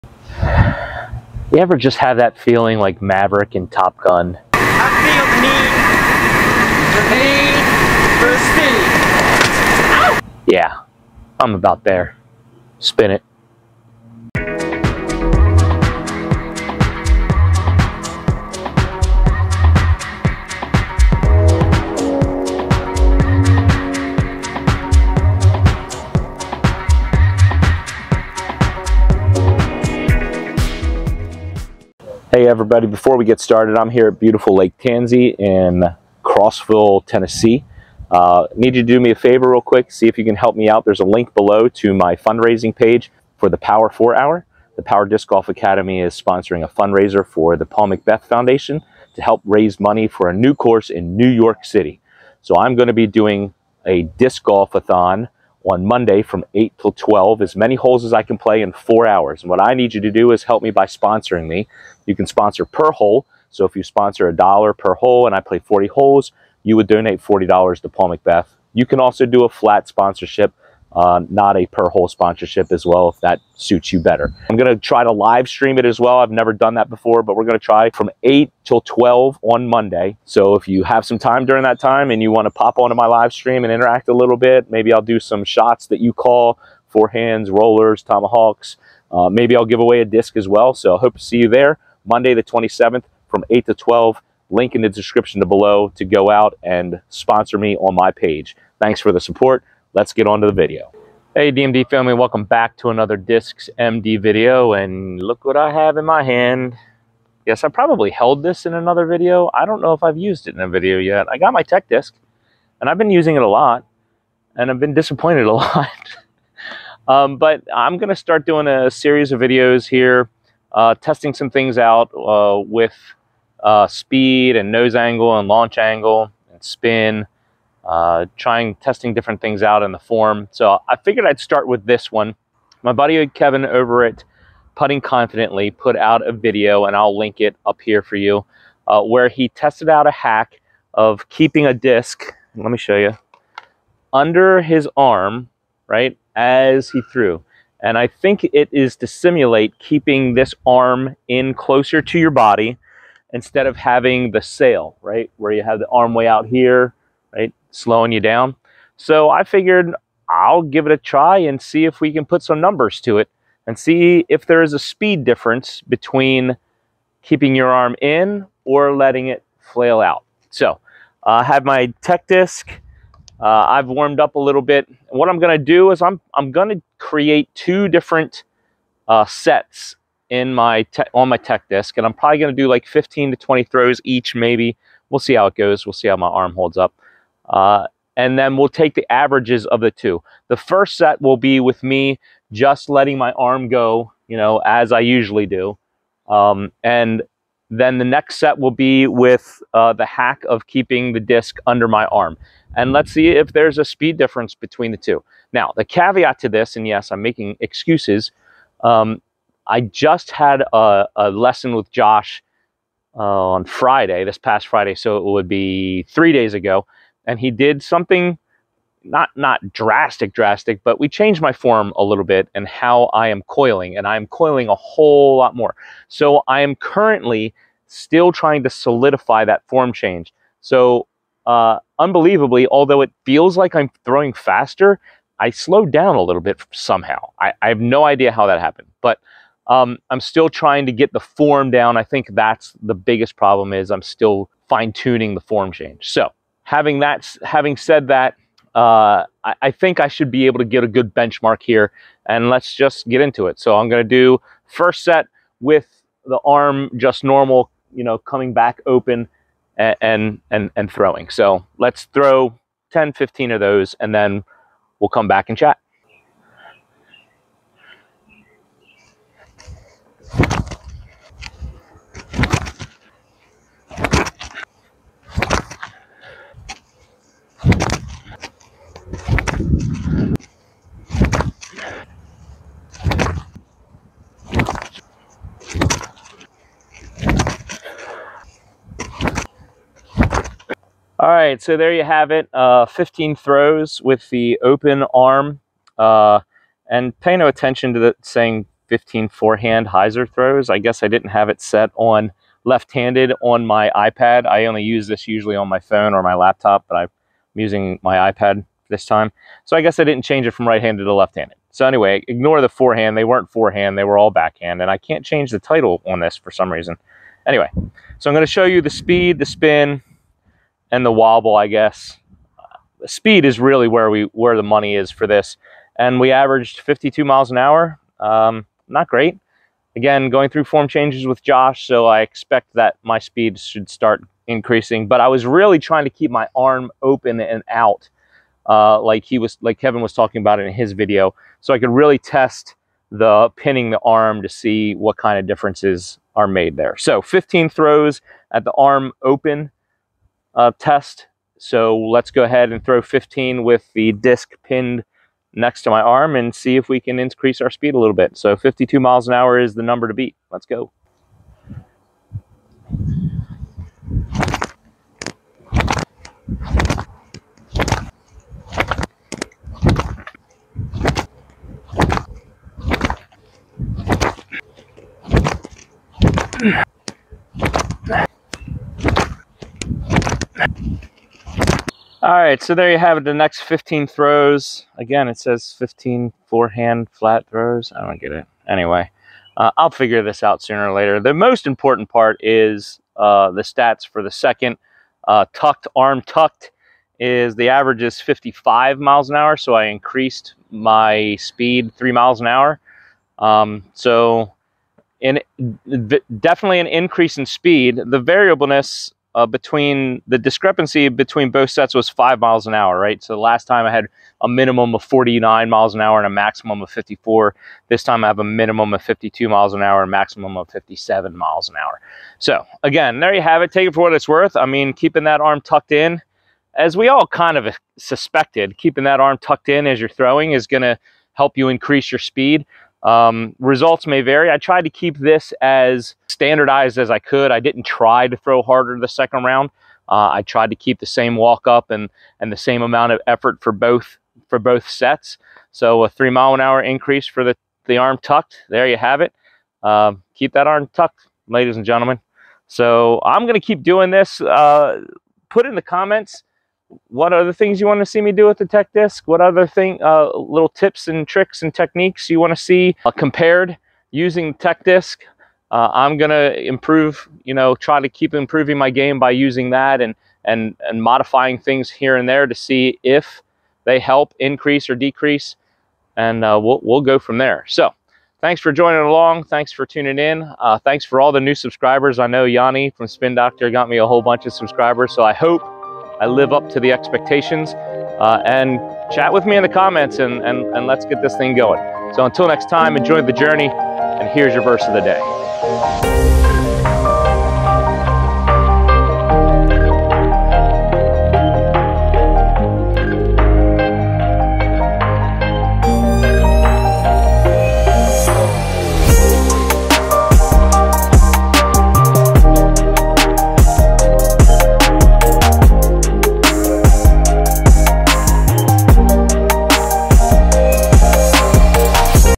How Much More Speed Does This Form Hack Generate? | Improving My Disc Golf Game
Can we add speed to our throw by using this form hack?  Thanks for the thought Kevin!

Support me as I raise money for the Paul McBeth Foundation on Monday May 27th from 8-12!!

Improve putting accuracy with Putting Confidently!
Use code - DISCSMD